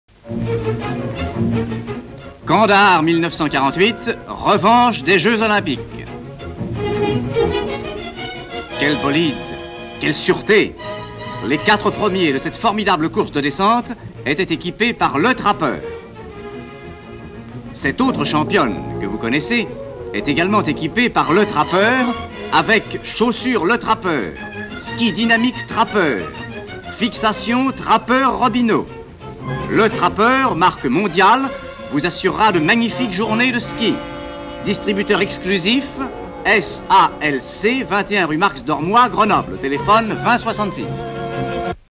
Film publicitaire 1948 : Le trappeur
Film de publicité de 1948 pour les équipements de ski Le Trappeur. Faire du slalom avec ce matériel c'est de l'exploit, aussi bien en 1948 qu' aujourd'hui. lorsque les films étaient réalisés, il y avait une copie de la publicité par ville avec l'adresse du magasin le plus proche. Les copies étaient donc nombreuses.